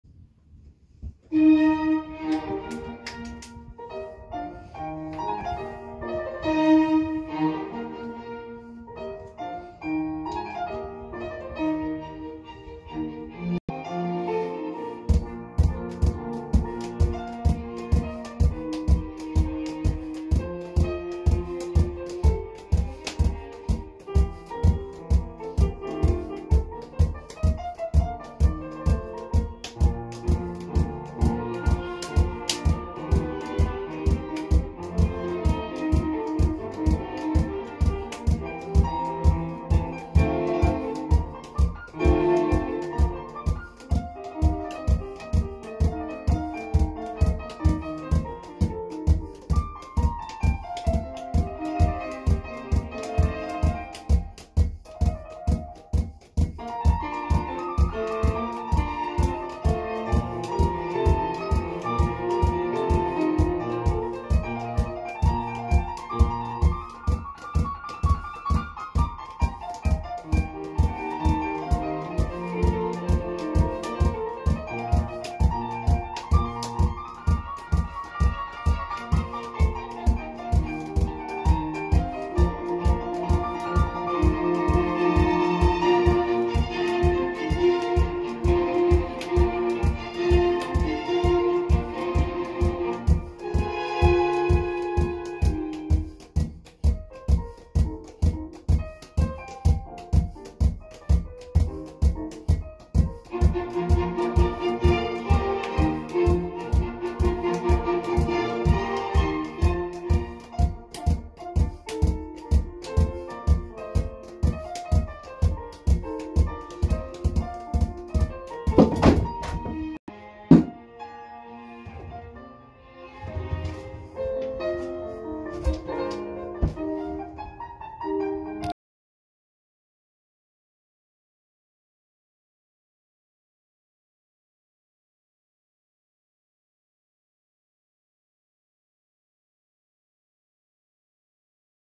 6-ball Force Bounce Juggling Slowest Minute Pattern 254c WR # 313
David Nayer matches the current 6-ball slowest force bounce juggling record with 254 catches. This considers the tallest pattern as measured by the fewest catches with a timed minute from when the ball leaves the first hand. Montage 220516 6bfb slow minute